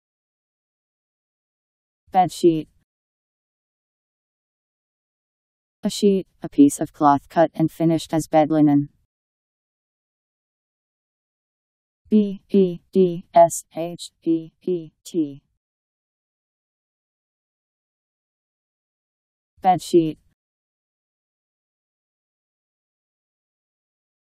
Bed sheet Meaning
Video shows what bed sheet means. A sheet, a piece of cloth cut and finished as bedlinen..  Bed sheet Meaning. How to pronounce, definition audio dictionary. How to say bed sheet. Powered by MaryTTS, Wiktionary